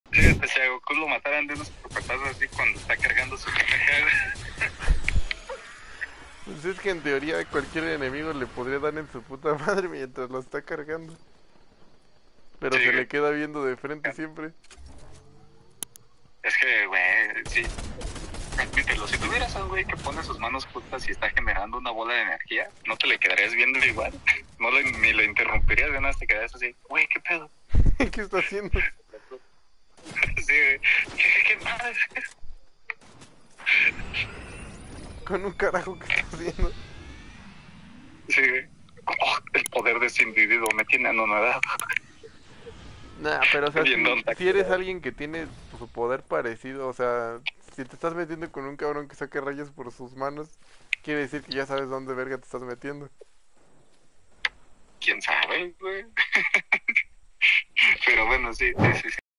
Goku vs Escopeta
En este corto Mike y Blaker nos dan su explicación de que pasaría si goku se enfrenta a una escopeta.